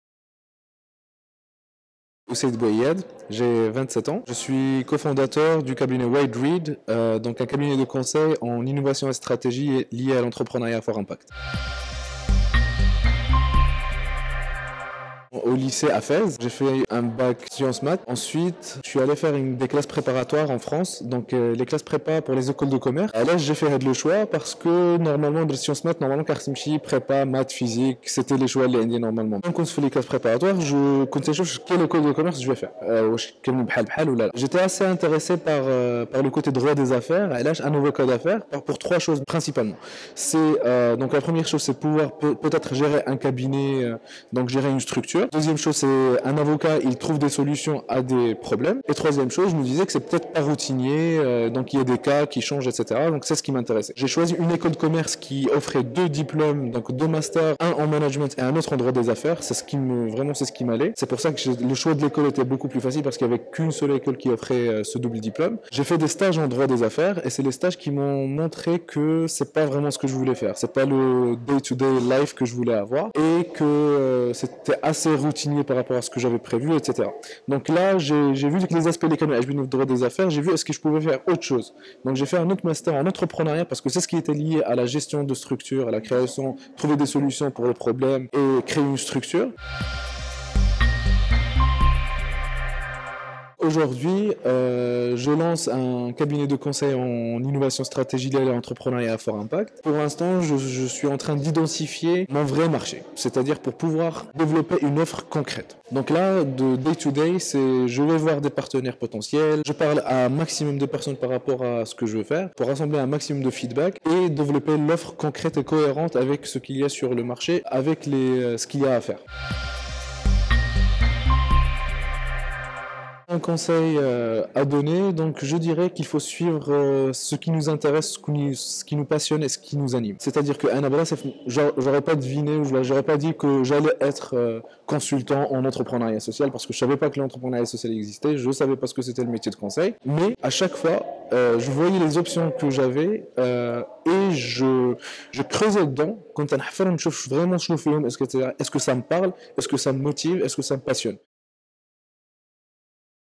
Le métier de consultant en entrepreneuriat : entre commerce et management !
Passionné par le métier du management et de commerce, Oussayd BOUAYAD a décidé de faire un master en droit des affaires et en management dans une grande école française. Après avoir effectué plusieurs stages dans ce domaine, Oussayd intègre le monde du travail  pour devenir aujourd'hui consultant. Aujourd'hui, Oussayd lance son cabinet de conseil et partage avec nous sa passion pour ce métier.